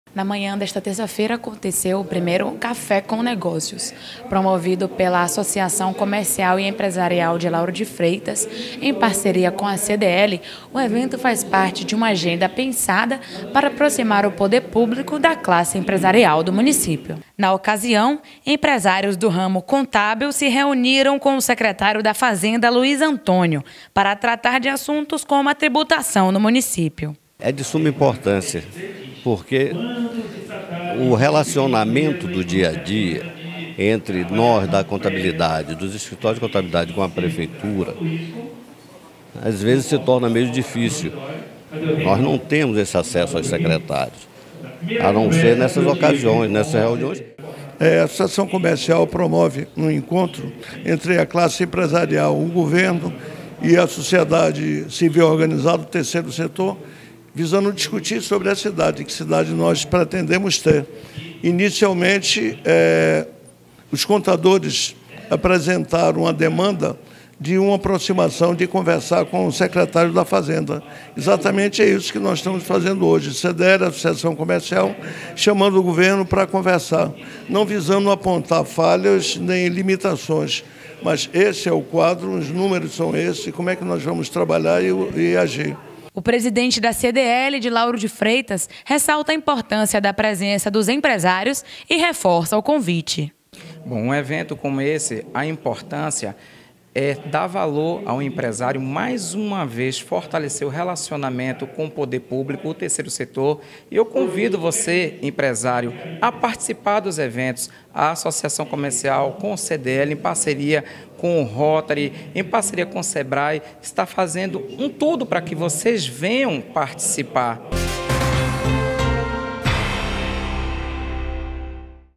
Café com Negócios
Na manhã de hoje a Associação comercial e empresarial em parceria com a CDL realizaram o primeiro Café com Negócios, ligando o poder público aos empresários do município.